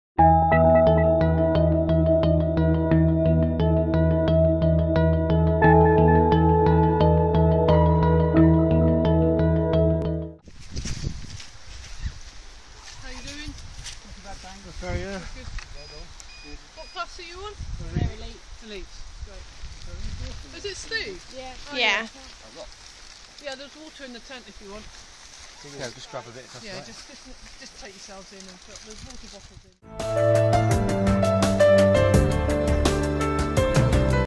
Marmot Dark Mountains - Steve Birkinshaw & Tom Gibbs
Leading Elite team Steve Birkinshaw & Tom Gibbs arrived at the Support Point